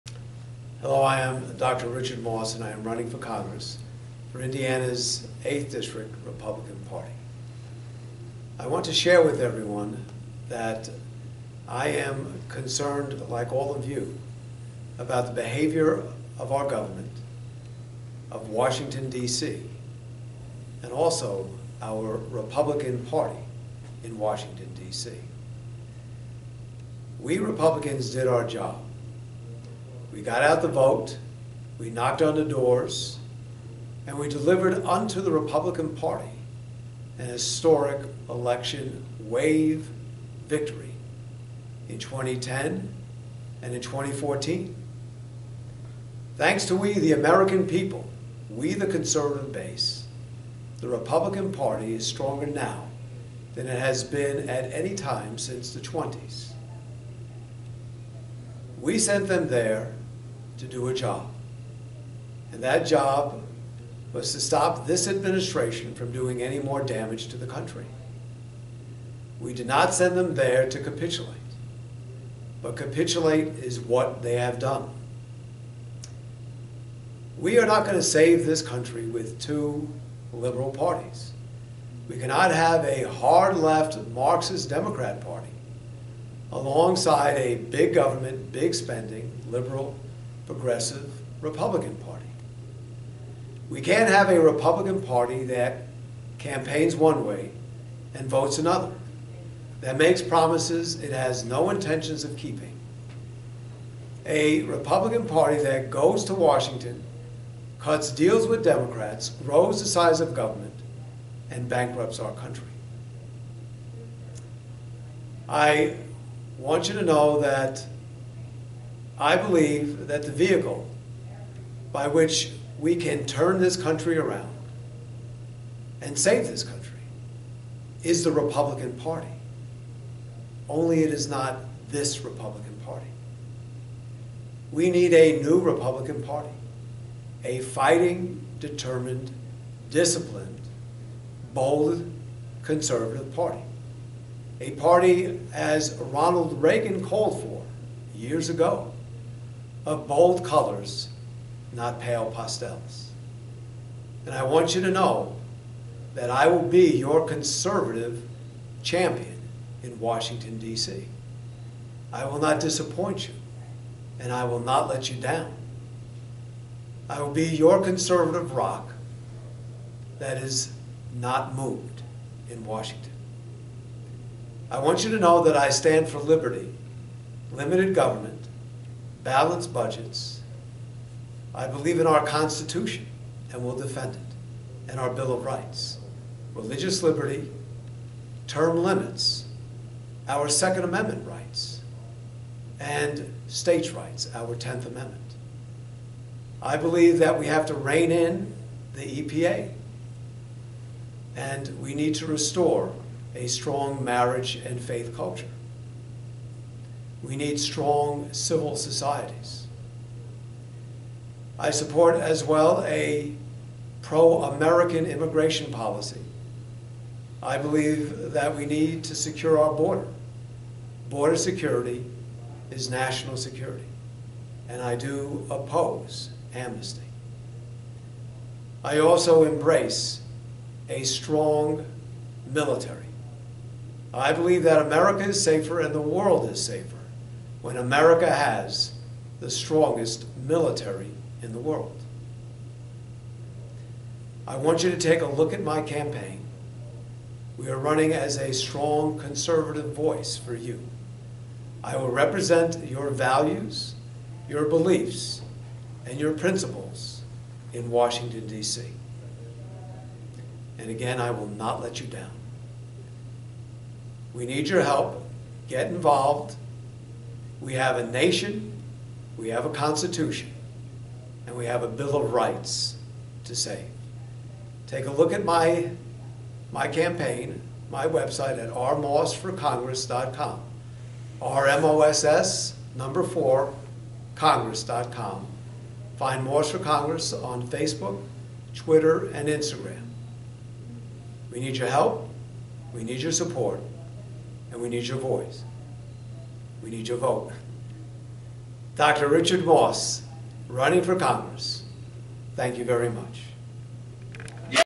The Failure of the Republican Establishment February 2016 Evansville IN
Dr. Moss explains the frustration that he, other Hoosiers, and millions of Americans across the nation with the Republican Party in Washington. They are out of touch and seem to be a war with their conservative base. We need a new, bold conservative Republican Party if we are to undo the damage of the Obama years and restore the nation to greatness.